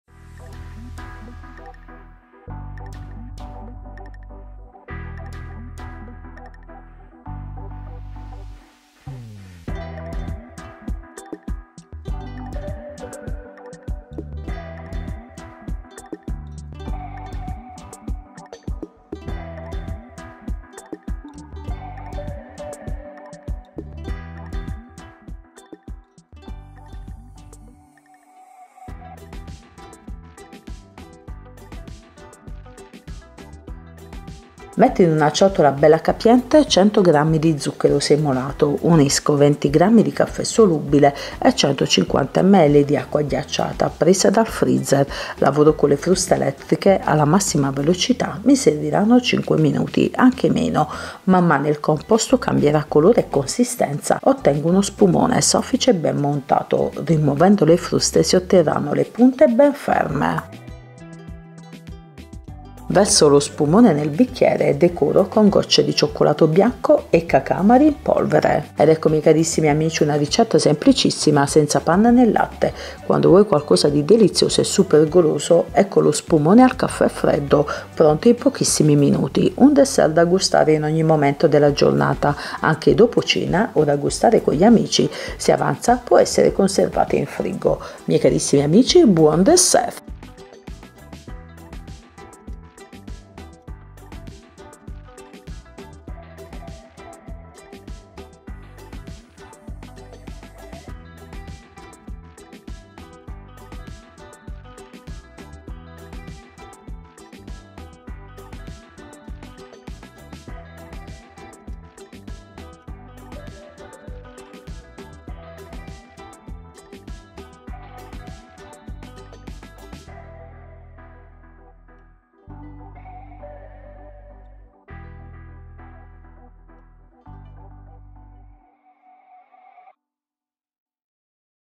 Crema al caffè: veloce e golosa! Un dolce cremoso e indimenticabile! SENZA LATTE NE PANNA!
Lo spumone al caffè è una ricetta molto facile da realizzare e non prevede l’uso della panna o latte, gli ingredienti sono solo 3: caffè solubile, acqua e zucchero!
Scorri per scoprire la ricetta!

🇮🇹 INGREDIENTI:🇮🇹
20 gr di caffè solubile di buona qualità
100 gr di zucchero semolato
150 ml di acqua ghiacciata
🇮🇹 INGREDIENTS:🇮🇹
20 gr of good quality instant coffee
100 grams of granulated sugar
150 ml of ice water
ZUTATEN:🇮🇹
20 g hochwertiger löslicher Kaffee
100 g Feingrieß von Zucker
150 ml eiskaltes Wasser
🇮🇹 INGRÉDIENTS:🇮🇹
20 gr de café instantané de bonne qualité
100 g de sucre cristallisé
150 ml d’eau glacée
INGREDIENTES:🇮🇹
20 gr de café instantâneo de boa qualidade
100 gramas de açúcar granulado
150 ml de água gelada
INGREDIENTE: B.B.B.B.B.B.C
20 gr de cafea instant de bună calitate
100 grame de zahăr granulat
150 ml de apă cu gheață
🇮🇹 INGREDIENTES:🇮🇹
20 gr de café instantáneo de buena calidad
100 gr de azúcar granulada
150 ml de agua helada
🇮🇹 ИНГРЕДИЕНТЫ:🇮🇹
20 г растворимого кофе хорошего качества
100 граммов гранулированного сахара
150 мл ледяной воды